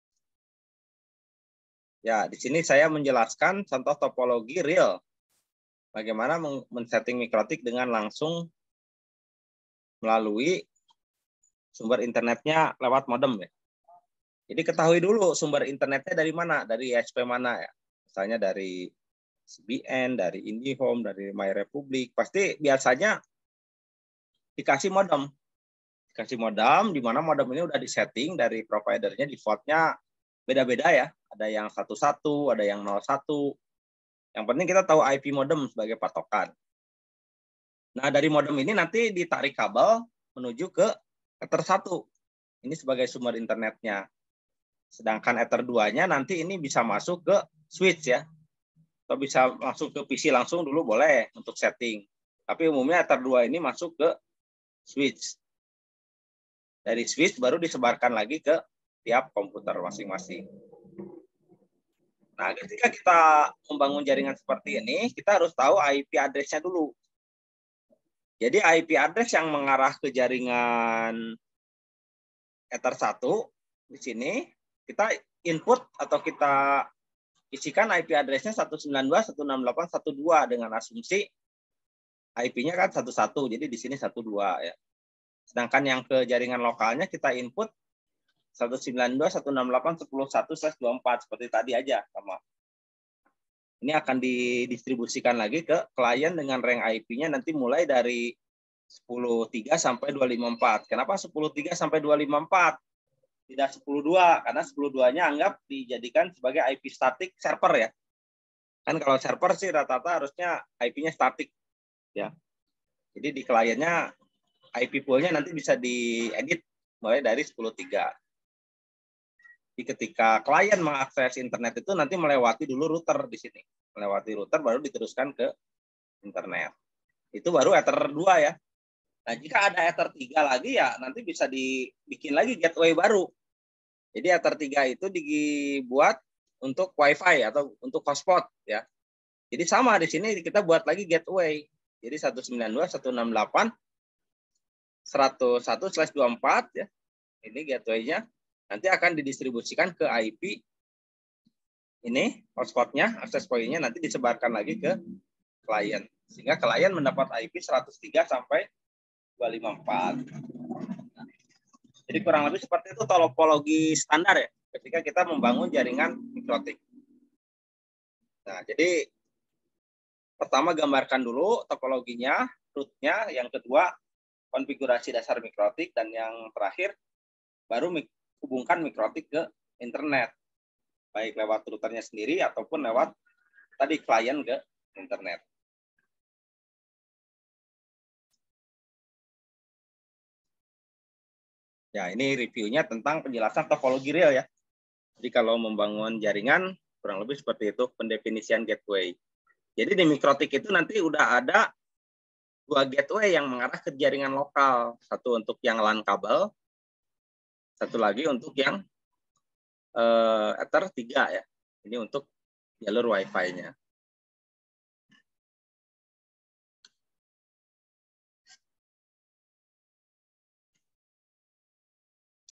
Penjelasan Topologi Jaringan Mikrotik || Topologi Dasar 1 ISP
1. Sumber Internet
2. Modem ISP
3. Router Mikrotik
4. Switch
5. User/client